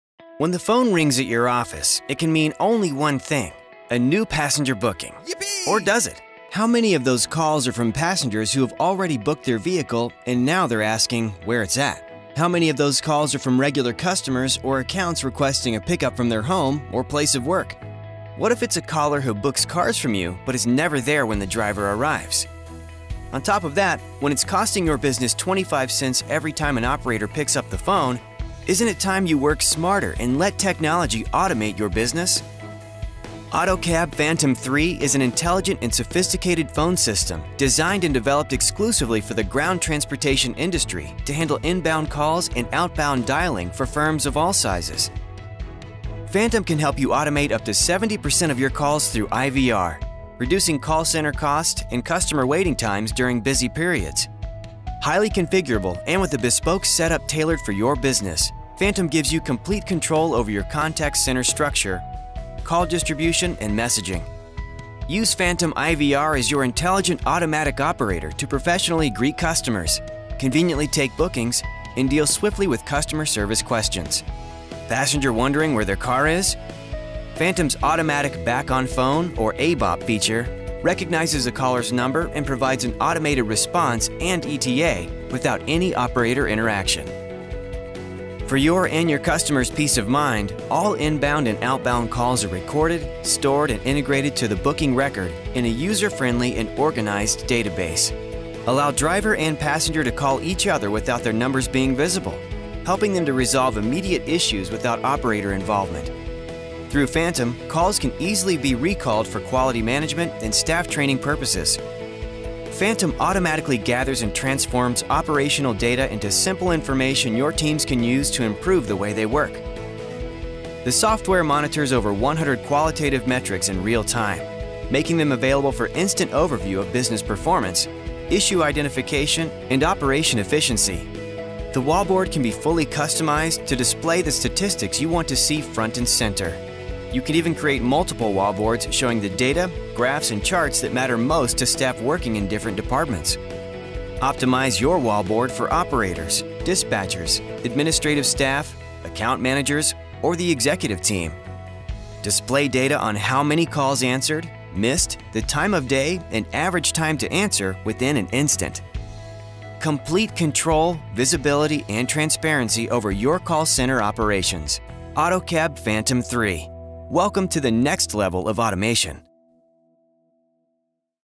Autocab Phantom - USA
Out-of-the-box automation

Autocab Phantom 3 is a highly intelligent telecommunication system specifically designed for the ground transportation industry. Its purpose is to maximize a taxi business’s call handling capabilities, by reducing missed calls and increasing the volume of incoming calls answered. From day one it can automate about half your calls.